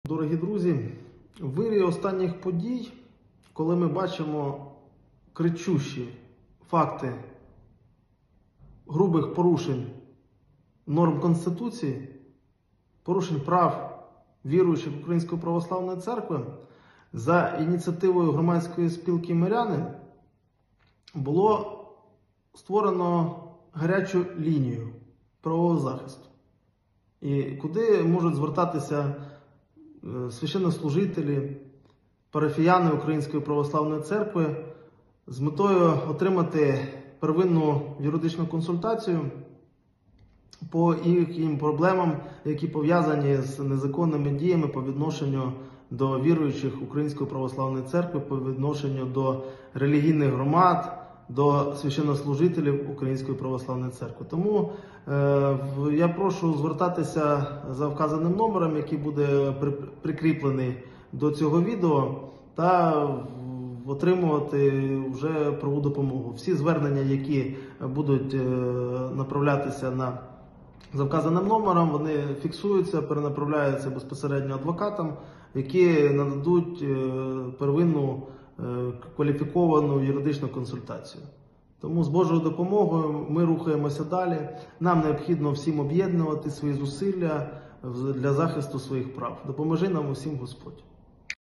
НАМ НЕОБХІДНО ОБ‘ЄДНУВАТИ НАШІ ЗУСИЛЛЯ ЗА ДЛЯ ЗАХИСТУ НАШИХ ПРАВ❗
Отець Микита Чекман, адвокат Лаври УПЦ, закликав православних адвокатів консолідувати зусилля для посилення юридичного фронту захисту вірян, духовенства та Святинь Української Православної Церкви.

⚖️Він також підтримав ініціативу ЦентрПравовогоЗахисту відкрити гарячу лінію для звернення всіх православних громадян України, які цього потребують

«Громадська спілка «МИРЯНИ» відкрила гарячу лінію правового захисту, куди можуть звертатися віряни та духовенства УПЦ. Я прошу звертатися за номером, який буде вказано під цим відео».

☝️За цим номером є можливість звертатися також через:
✅ Viber